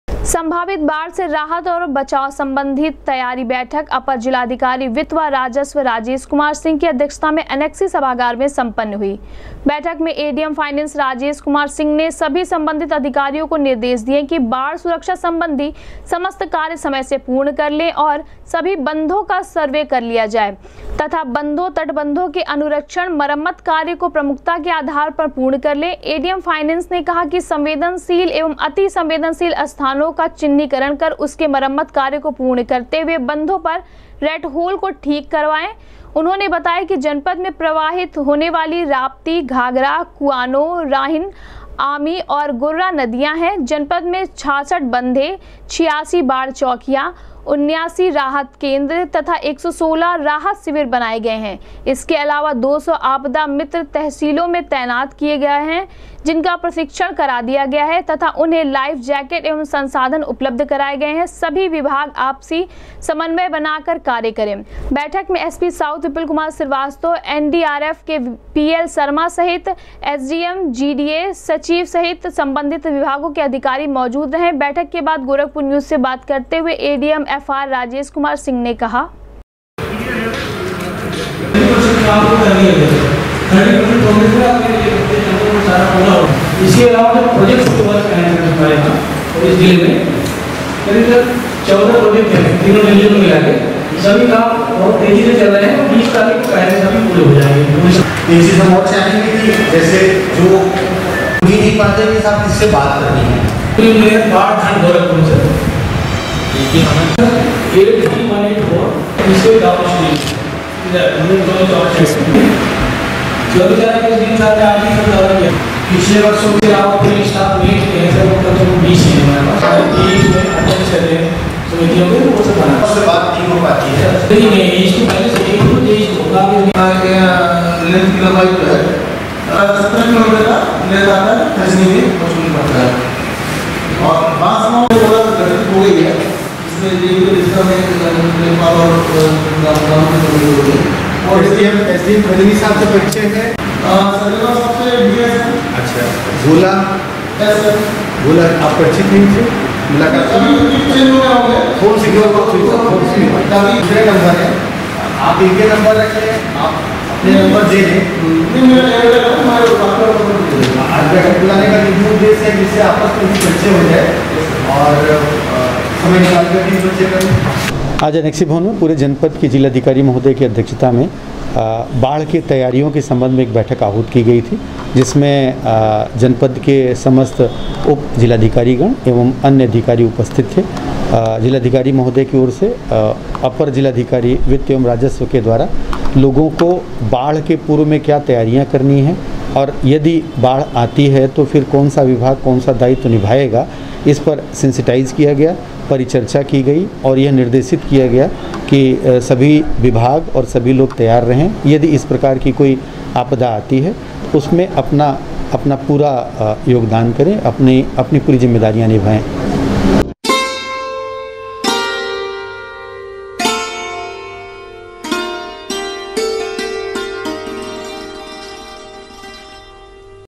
अपर जिलाधिकारी की अध्यक्षता में सम्पन्न हुई,सम्भावित बाढ़ से राहत और बचाव की तैयारी
अपर जिलाधिकारी की अध्यक्षता में सम्पन्न हुई,सम्भावित बाढ़ से राहत और बचाव की तैयारी बैठक,कहा समय से पहले कर ले बाढ़ से निपटने की तैयारी, बंधों का भी सर्वे कर उसका करा लें मरम्मत